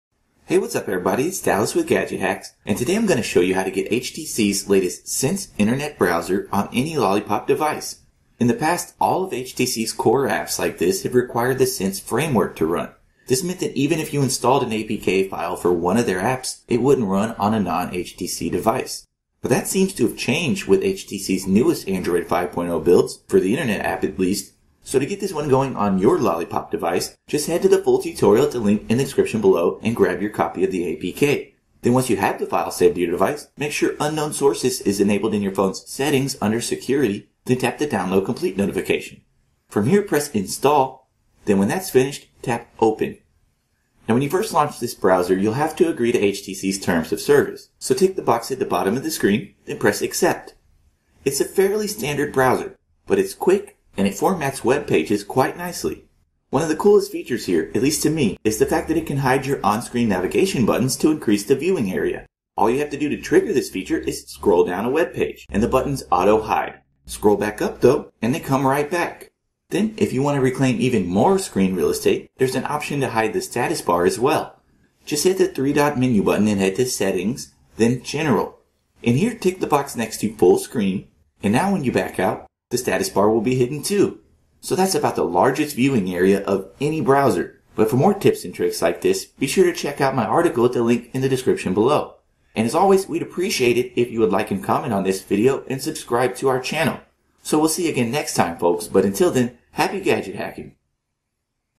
Get HTC's Sense Web Browser on Any Lollipop Device [How-To]
How to Install the HTC Browser on Your Android 5.0+ Device

In this tutorial, I'll be showing you how to install the newest version of HTC's Sense internet browser (Android 5.0+ required). With auto-hiding navigation buttons and an option to hide the status bar, this browser packs more information on the screen than any other.